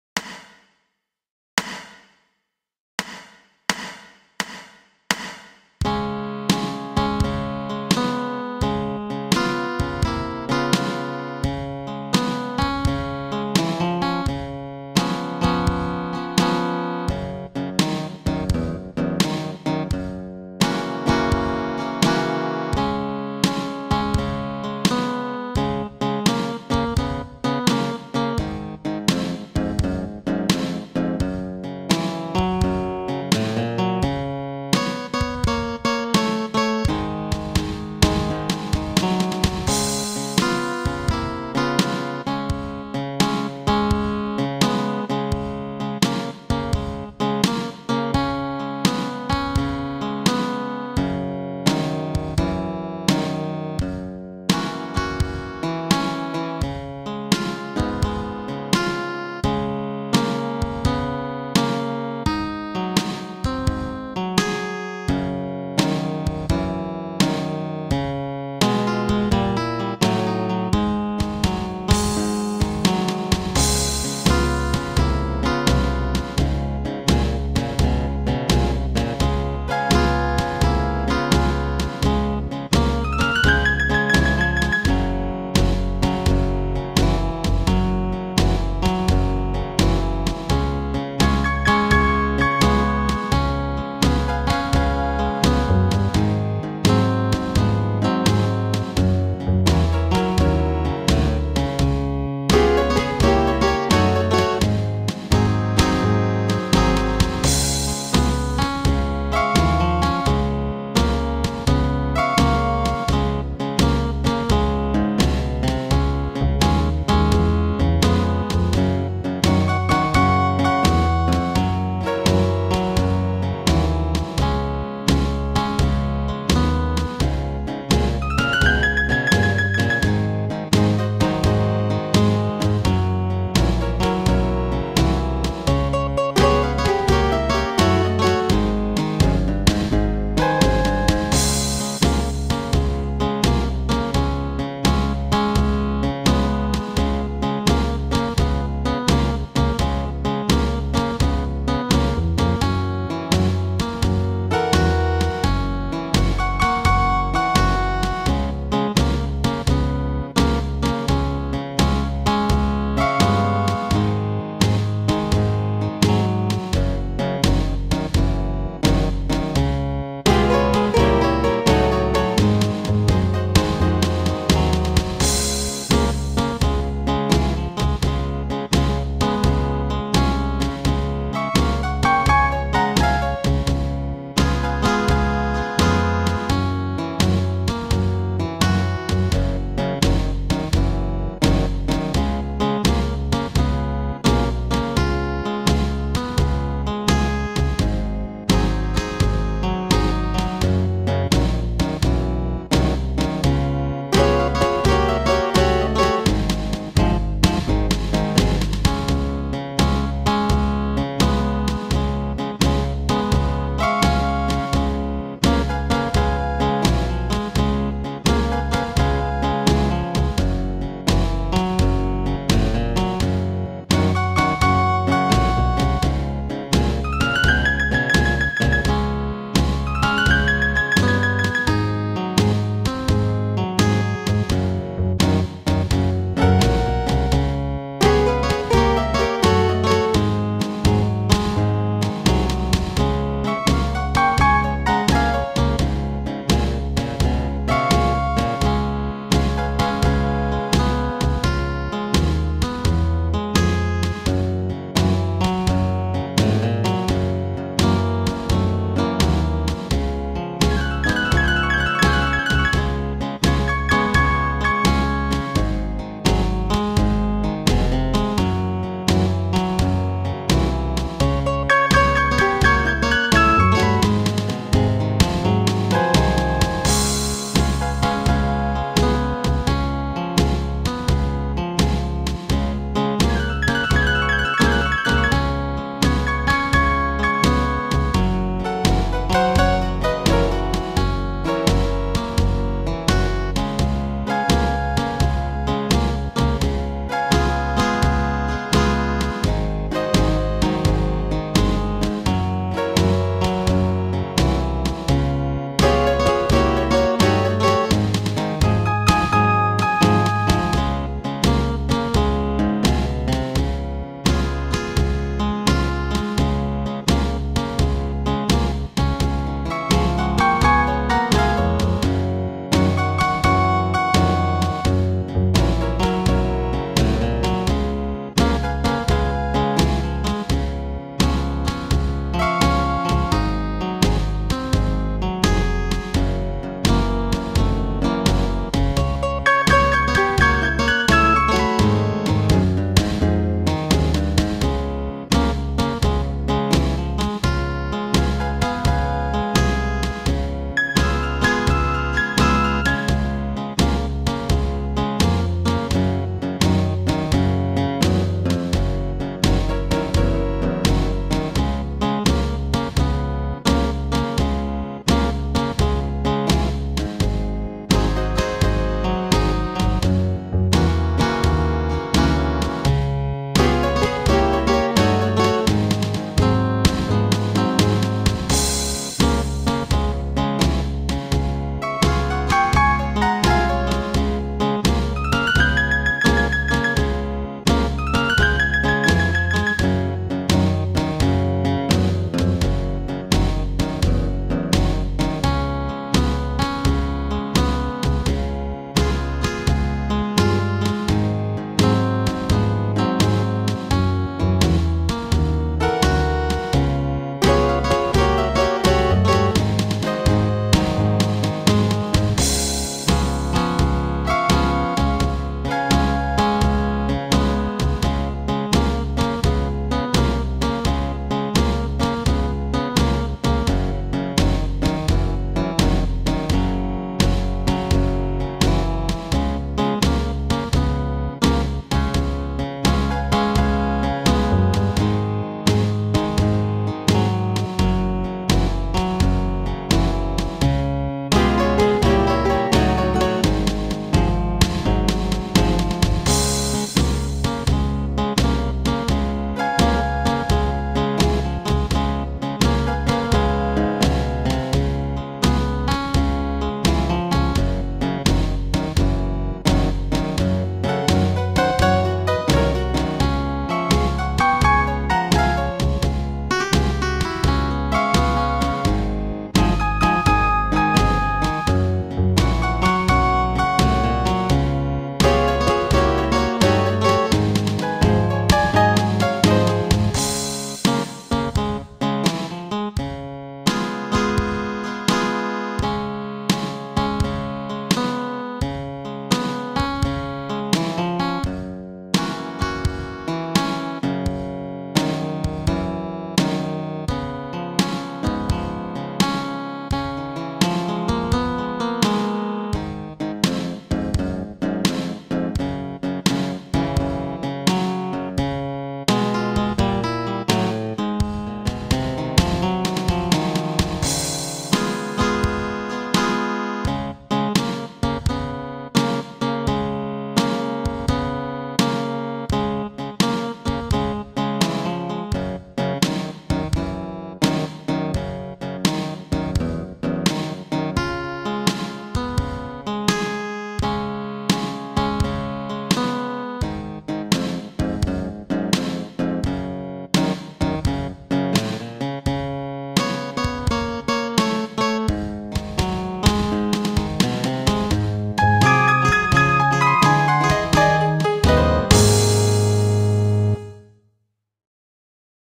Blues en C# para Improvisar - por Jesús Amaya...
Jesús Amaya nos muestra una pista de Blues para improvisar...